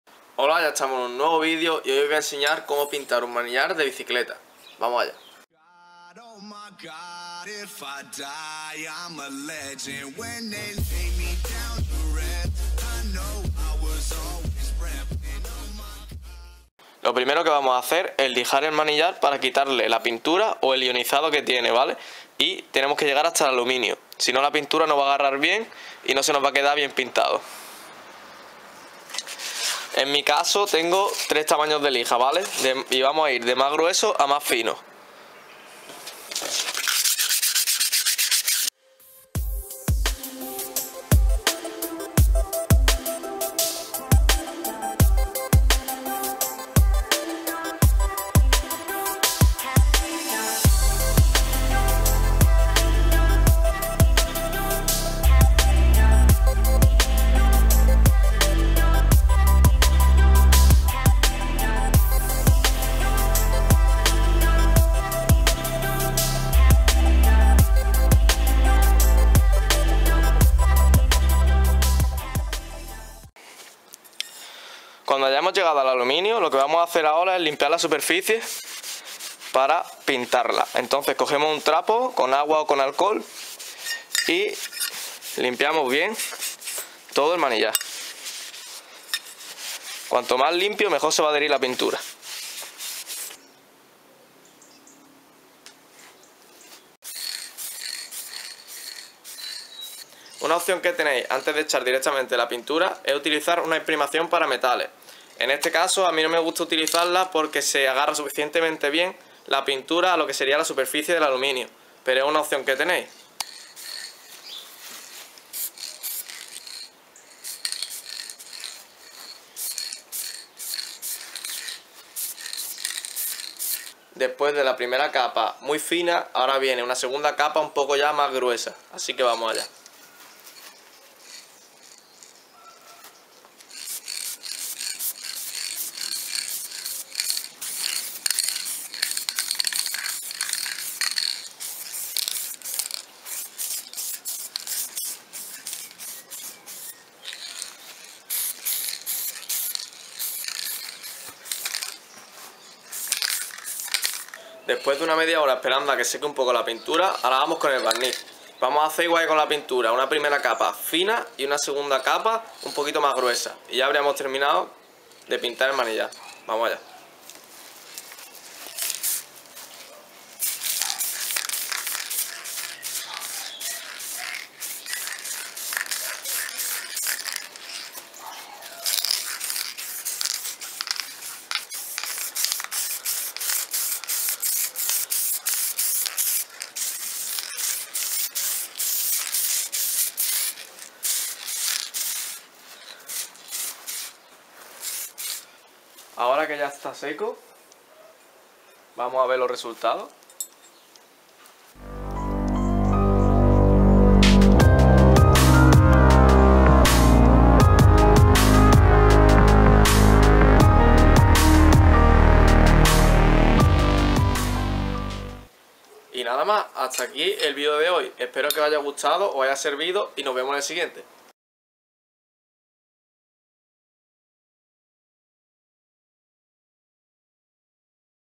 COMO PINTAR UN MANILLAR DE BICICLETA
Después del vídeo donde os mostré como pinte mi casco, aquí viene una segunda etapa con un mejor acabado.